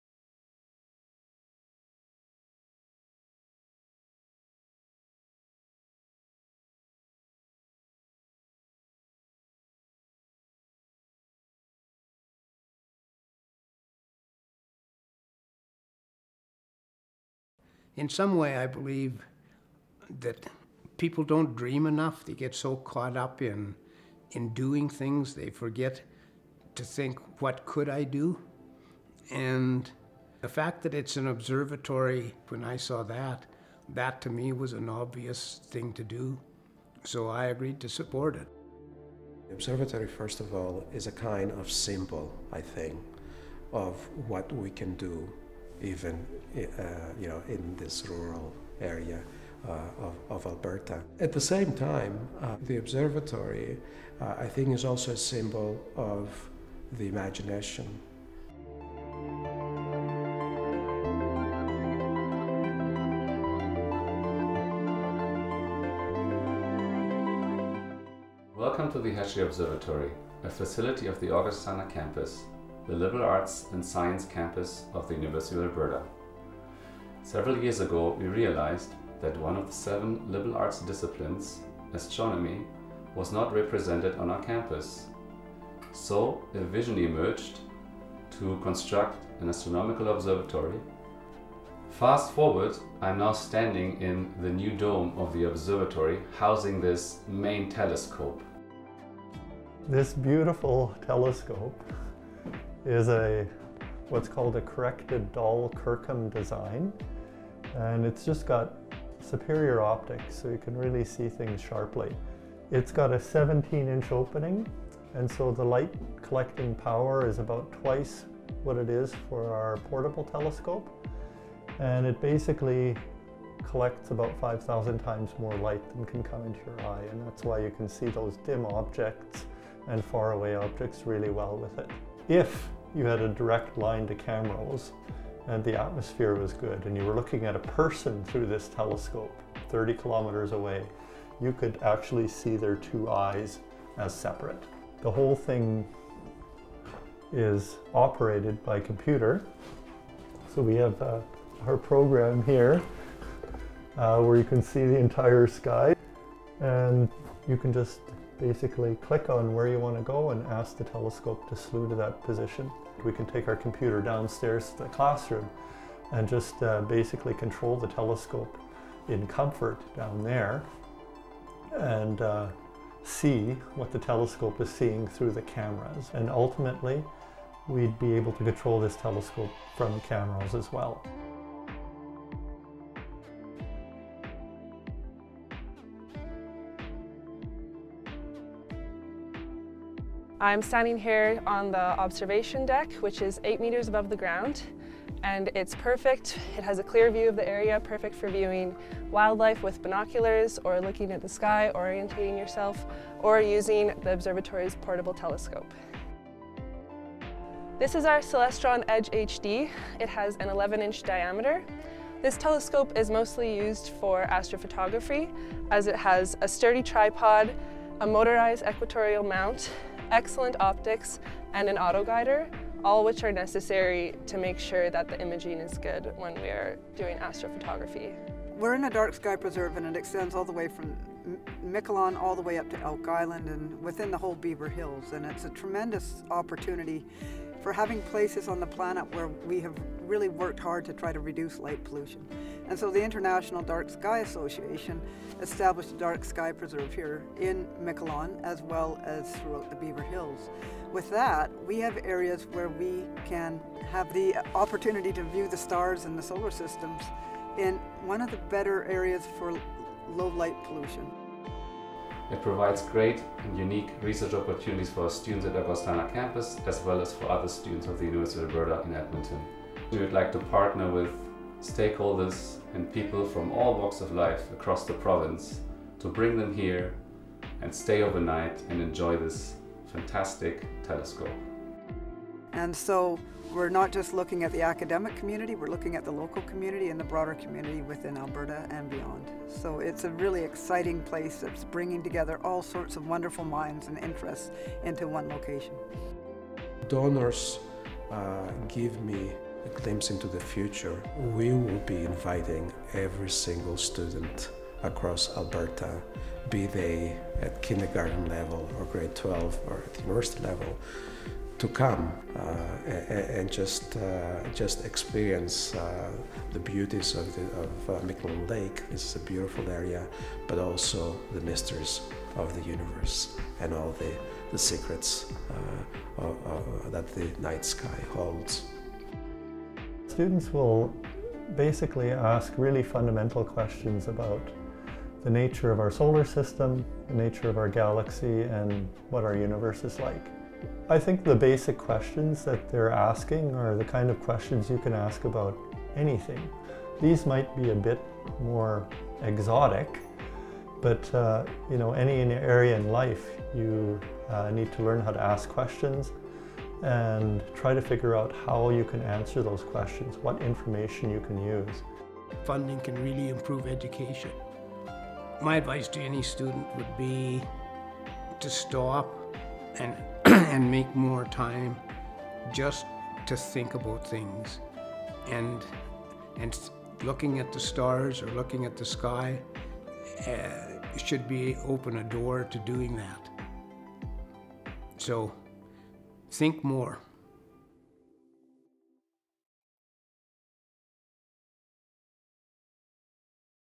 Hesje Observatory Launch Video
The University of Alberta Augustana Campus' Hesje Observatory at Miquelon Lake Provincial Park is a path to the stars for the public and students of all ages.

Although University of Alberta buildings are currently closed due to COVID-19 restrictions, we are looking forward to a time, post pandemic, when we can share this wonderful facility.

In order of appearance:
Brian Hesje, alumnus and donor
Peter Berg, Professor of Mathematics & Physics and Department of Science Chair
Demetres Tryphonopoulos, Augustana Faculty Dean
Gerhard Lotz, Associate Professor of Mathematics & Physics
Rae Metrunec, Research Assistant
Glynnis Hood, Professor of Environmental Science and Station Manager

Videographer: Daron Donahue of Donahue Productions

Director: Trina Harrison, Alumni & Donor Relations Coordinator

Editor: Sydney Tancowny, Advancement Communications Coordinator